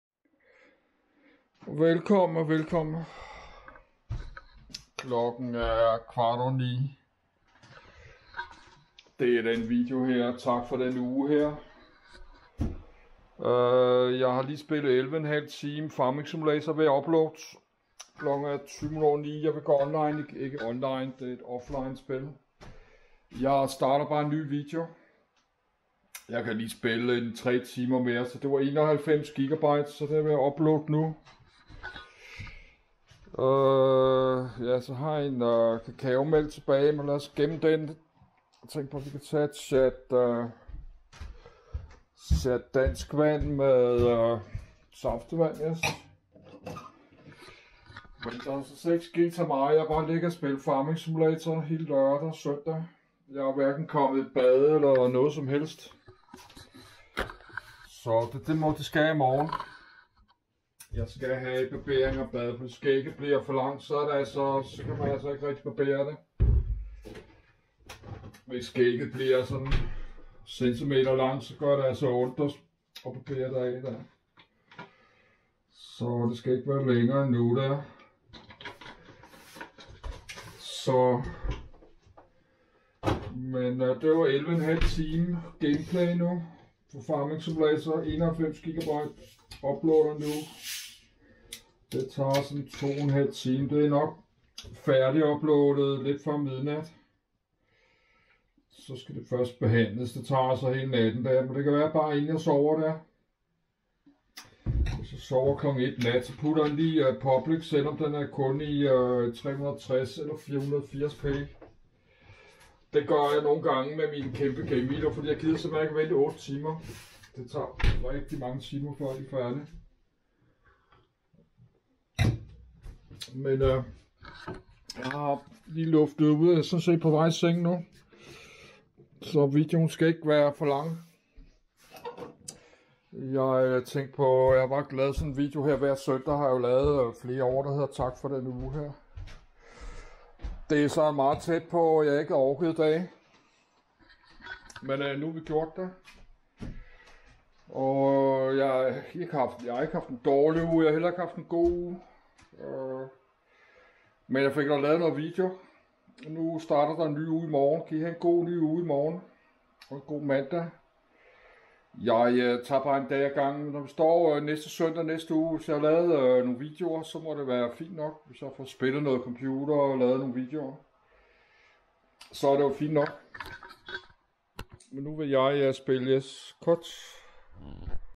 MongoTV_6113 - TAK FOR DENNE UGE
Kom godt igennem SØNDAG NAT. Så gik der en uge til. Jeg har ikkke haft en dårlig uge, den har bare været sådan  OK. Jeg fik da optaget en masse video igen, og det er snart rigtig lunt forår. Så det må bare være godt nok for nu. Klokken den er 21.30 om lidt, og jeg har lige spillet Farming Simulator 19 i 11 1/2 time, og jeg ved at uploade den 91 GB game video nu. Den kan jeg måske lige når at gøre offentlig, omkring klokken 1 nat. Jeg starter en ny video nu, og spillet Farming Simulator 19 en 3-4 timer mere nu. Godnat.